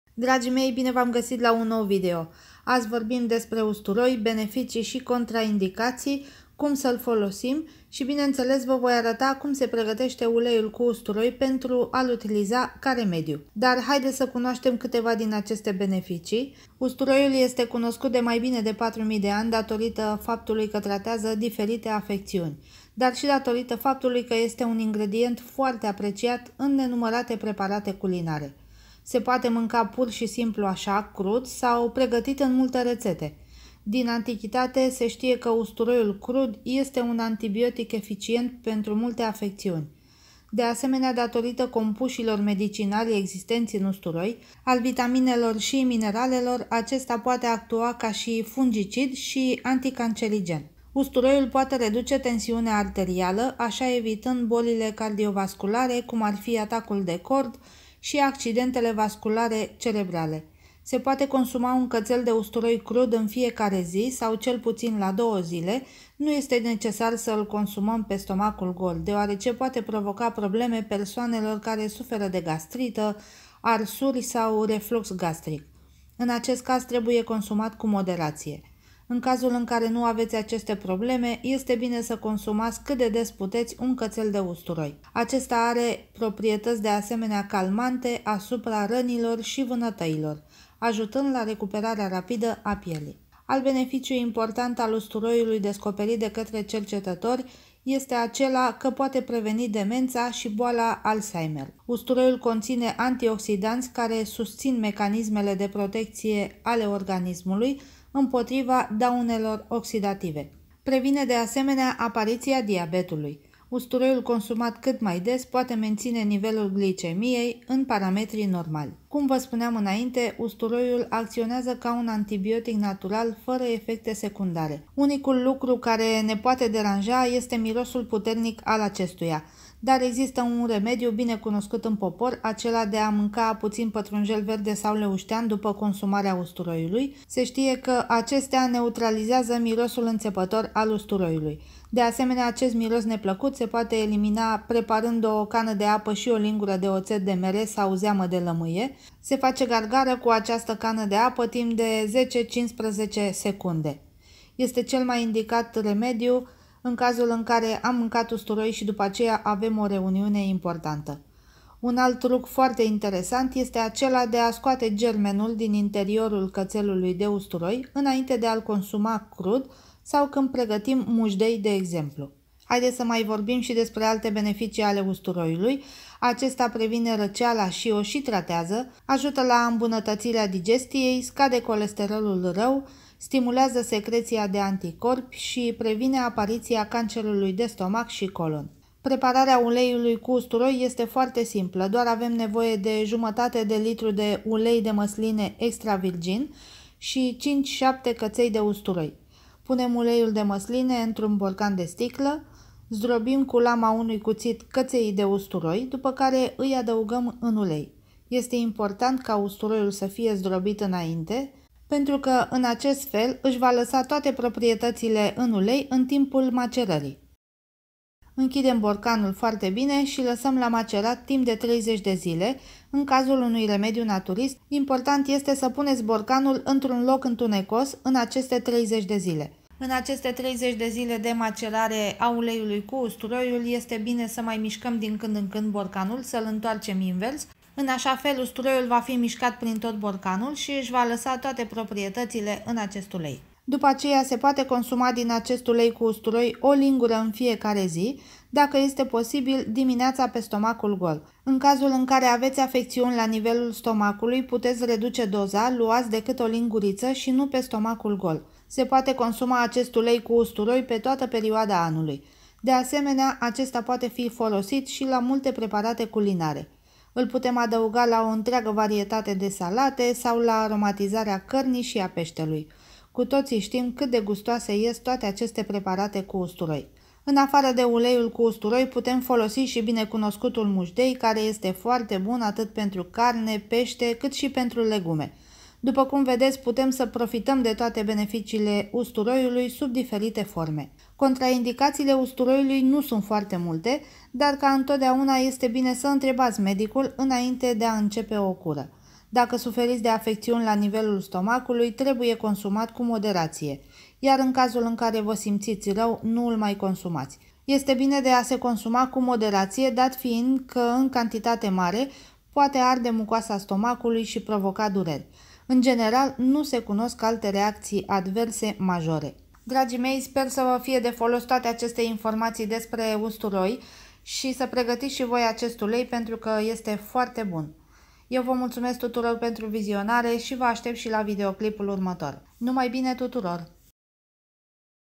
Usturoiul, beneficii si contraindicatii + Reteta de ulei cu usturoi pentru multe afectiuni
In acest video vorbim despre usturoi ca remediu natural pentru multe afectiuni. De asemenea vom pregati si un ulei cu usturoi ideal pentru tratarea multor afectiuni si pentru aromatizarea mancarurilor. Urmariti videoul pana la final pentru a afla toate beneficiile usturoiului.